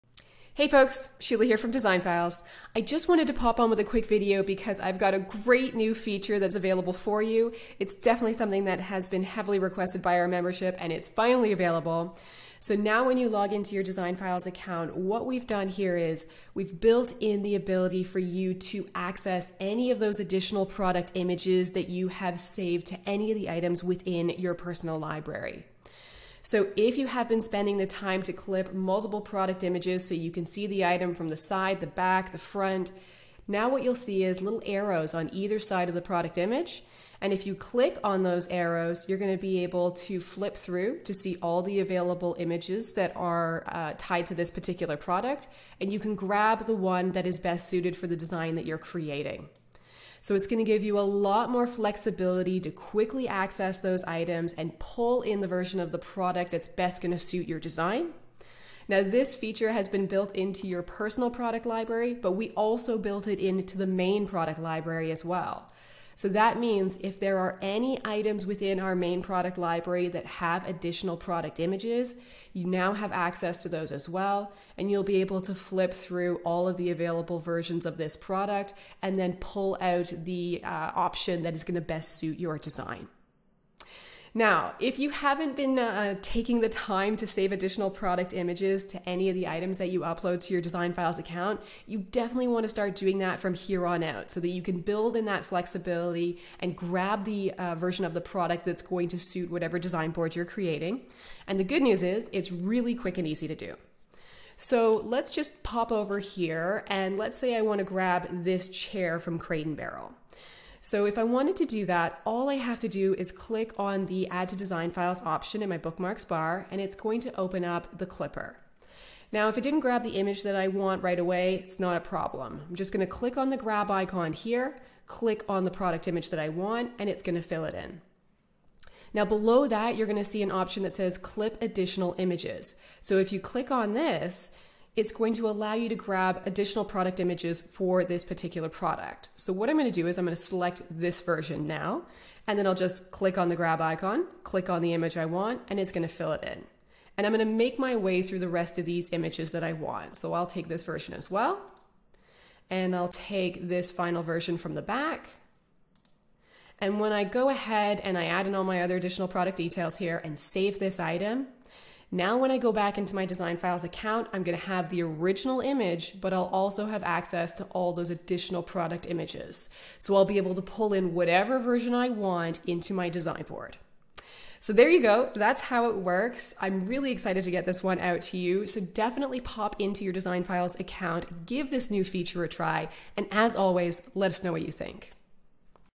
DesignFiles.co - Use Additional Images in the Editor
Easily access and use alternate product images (such as different angles, colors, or views) that are saved in your product library. Toggle between images to select the best fit for your interior design boards.